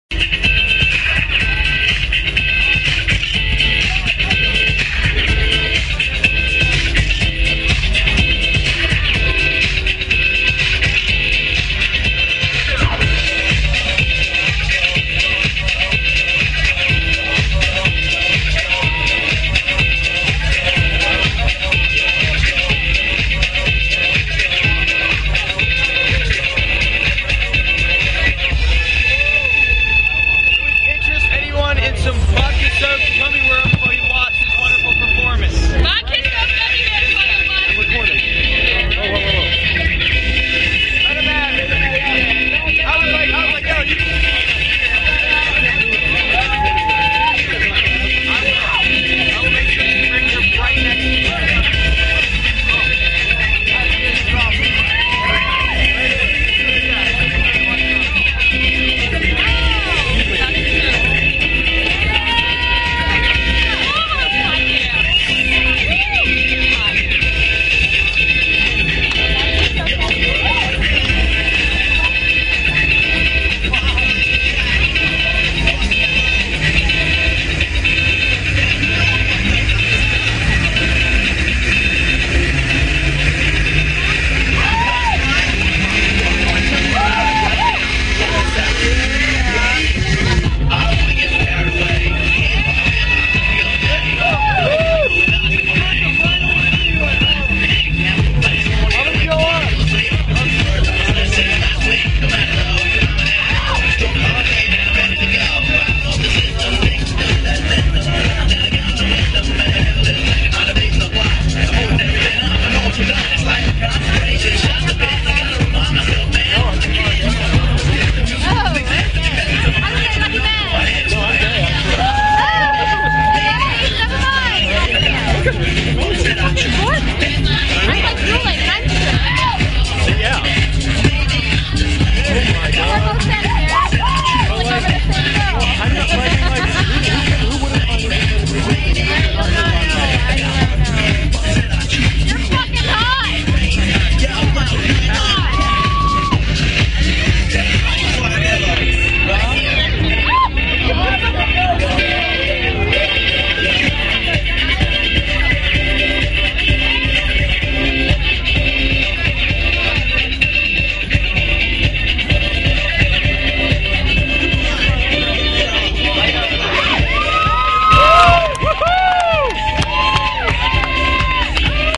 Kira Contortions Pole-dancing at PDF 2011
Pole-dancing is even cooler under black light than it usually is... and cooler still when the dancer is a professional acrobat.